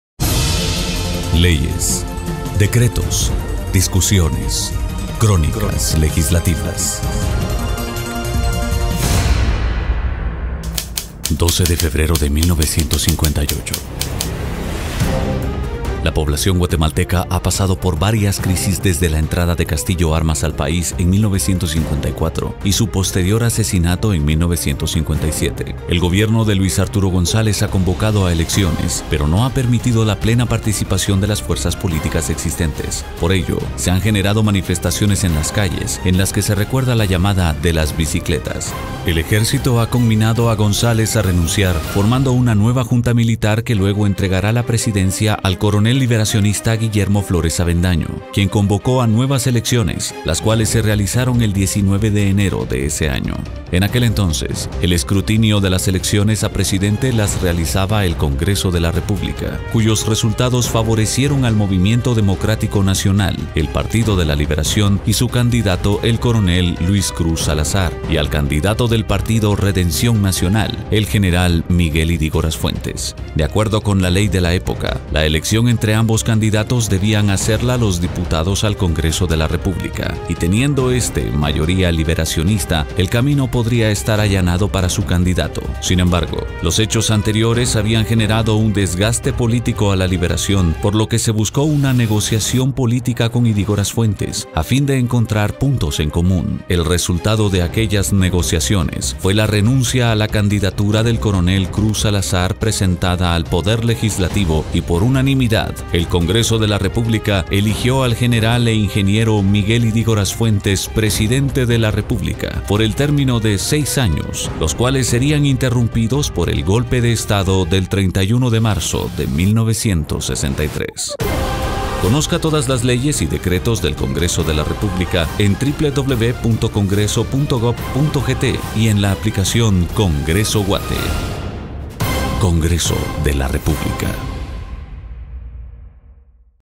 CRÓNICAS LEGISLATIVAS, MIGUEL YDÍGORAS
Serie de micro documentales elaborados para el Canal 9 del Organismo Legislativo de Guatemala. Guión y locución Oscar Monzón,  Edición Carlos Ventura R.